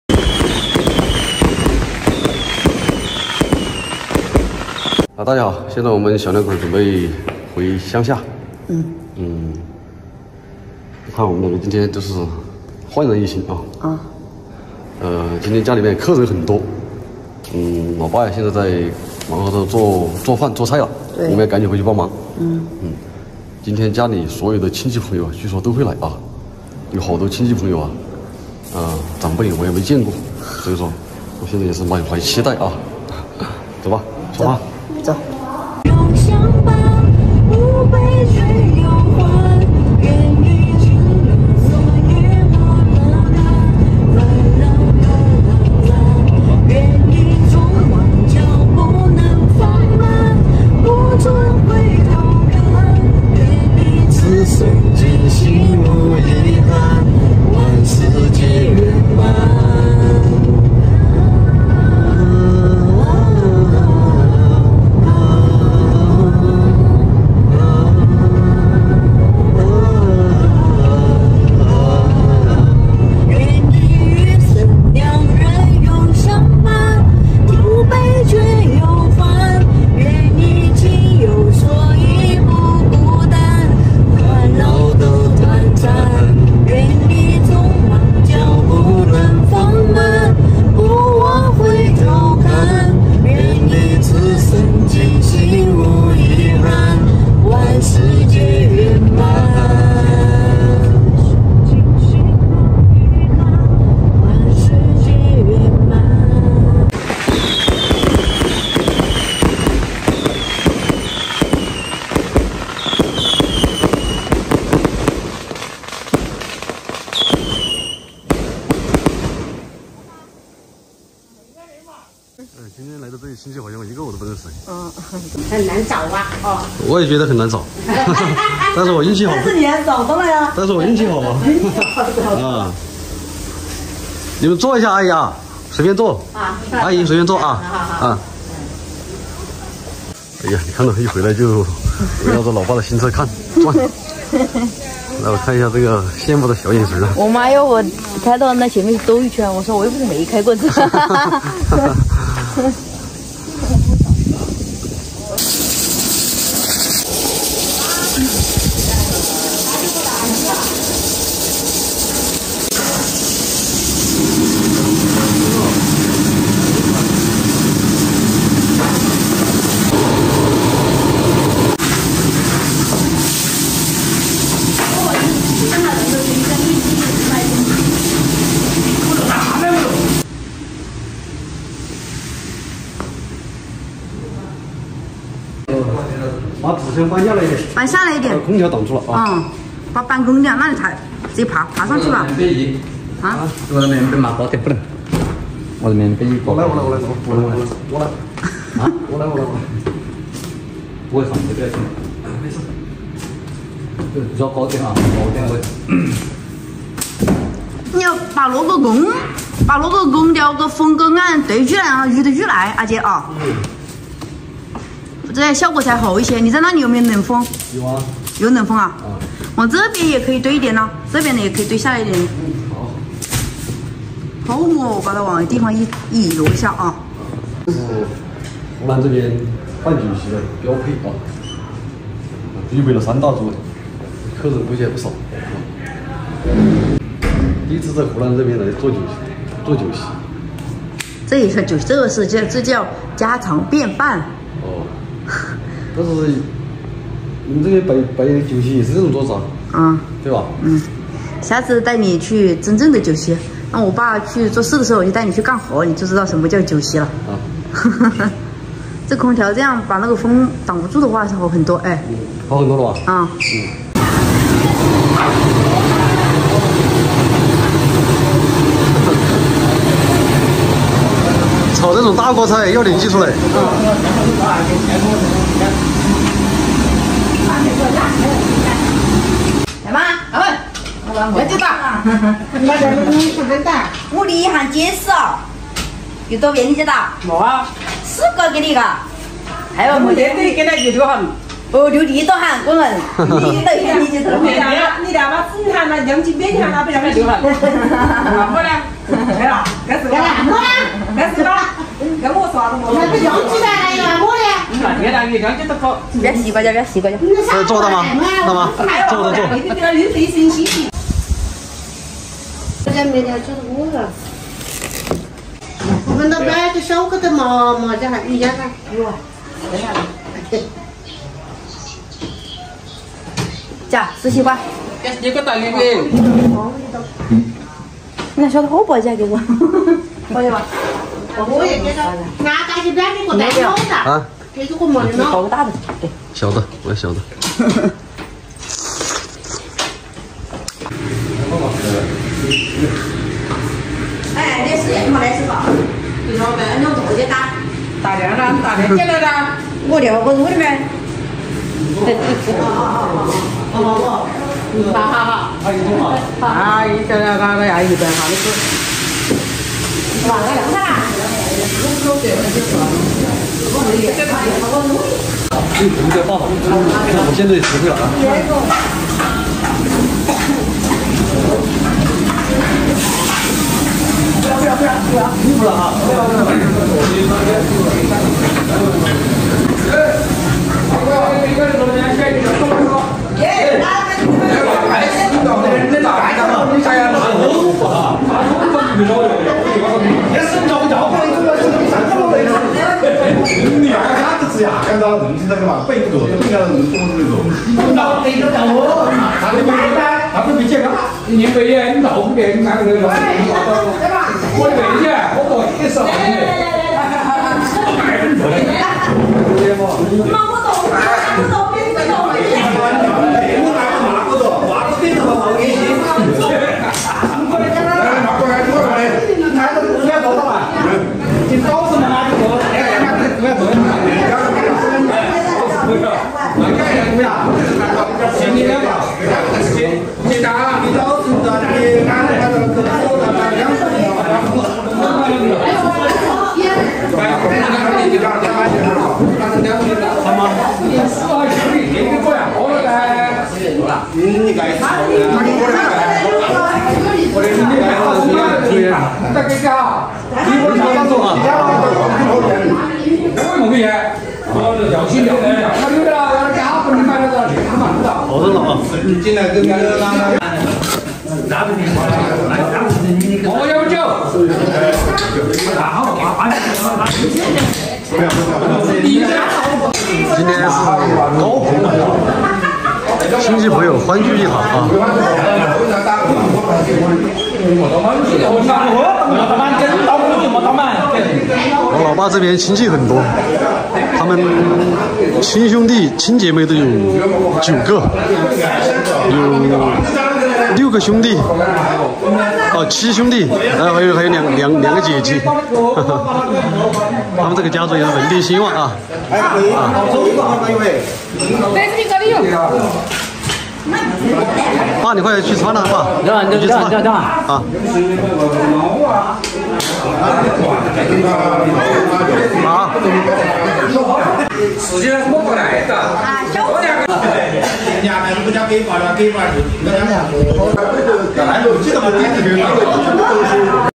今天家裡太熱鬧，親朋好友都來了，足足坐滿三大桌#農村
请放心，我们的视频旨在教育如何安全、合法地处理害虫问题，无任何动物受害。 哈嘍~大家好，這裡是【土家阿傑】YouTube官方正版頻道